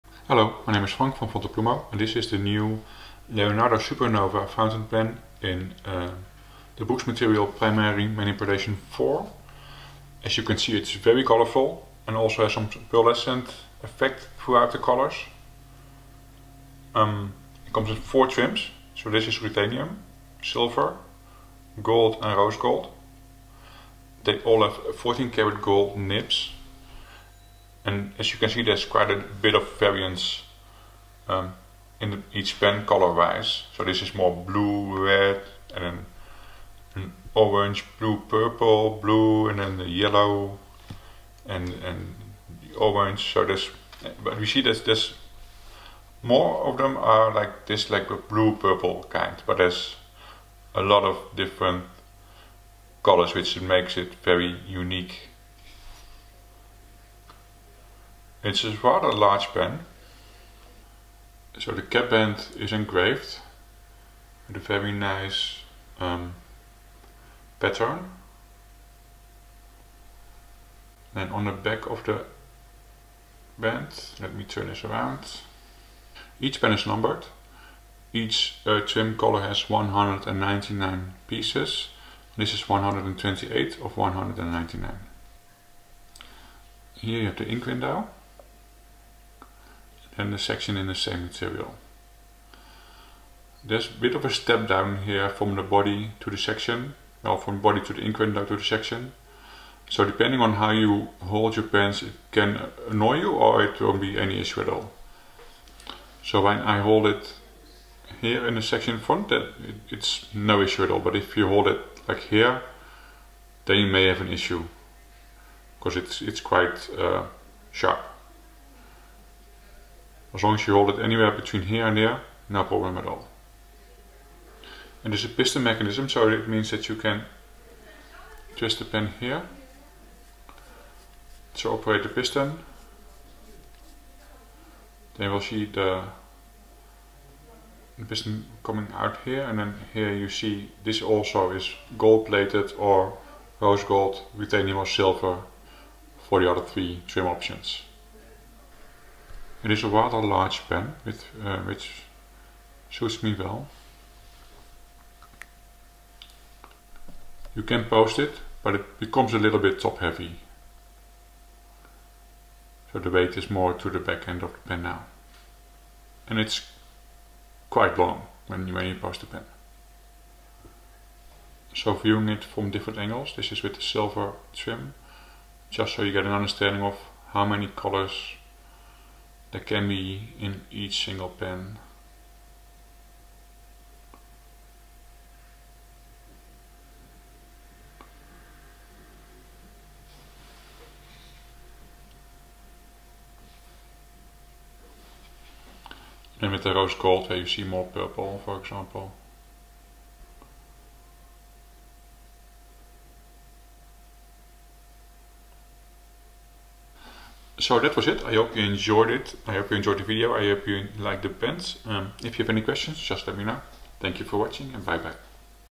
Leonardo Supernova fountain pens in Primary Manipulation #4 resin
A closeup of the Leonardo Supernova fountain pens in Primary Manipulation #4 resin by the Carolina Pen Company.
These fountain pens are limited to only 199 pieces per each trim color: Silver, Gold, Rose Gold and Ruthenium.
Each pen is numbered on the bottom of the body.